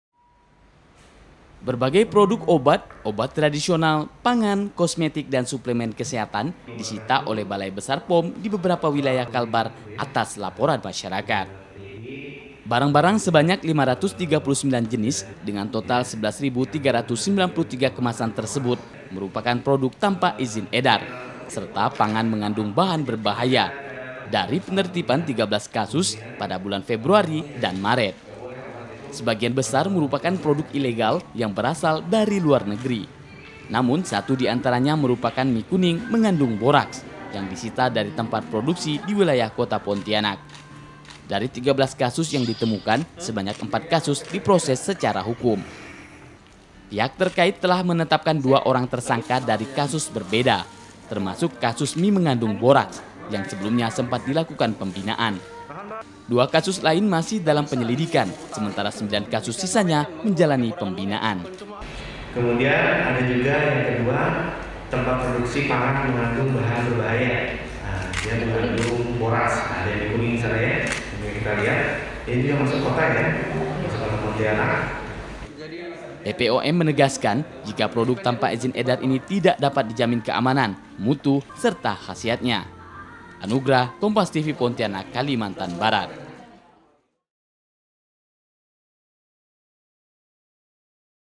BBPOM di Pontianak Sita Produk Ilegal dan Berbahaya, Satu di Antaranya Mi Boraks
PONTIANAK, KOMPAS.TV - Berbagai produk obat, obat tradisional, pangan, kosmetik, dan suplemen kesehatan, disita oleh Balai Besar POM di beberapa wilayah Kalbar atas laporan masyarakat.

Barang-barang sebanyak 539 jenis dengan total 11.393 kemasan tersebut, merupakan produk tanpa izin edar, serta pangan mengandung bahan berbahaya, dari penertiban 13 kasus, pada bulan Februari dan Maret.

Sebagian besar merupakan produk ilegal yang berasal dari luar negeri, namun satu di antaranya merupakan mi kuning mengandung boraks, yang disita dari tempat produksi di wilayah Kota Pontianak. Dari 13 kasus yang ditemukan, sebanyak empat kasus diproses secara hukum.

Pihak terkait, telah menetapkan dua orang tersangka dari dua kasus berbeda, termasuk kasus mi mengandung boraks yang sebelumnya sempat dilakukan pembinaan. Dua kasus lain masih dalam penyelidikan, sementara sembilan kasus sisanya menjalani pembinaan.

BBPOM menegaskan, jika produk tanpa izin edar ini tidak dapat dijamin keamanan, mutu, serta khasiatnya.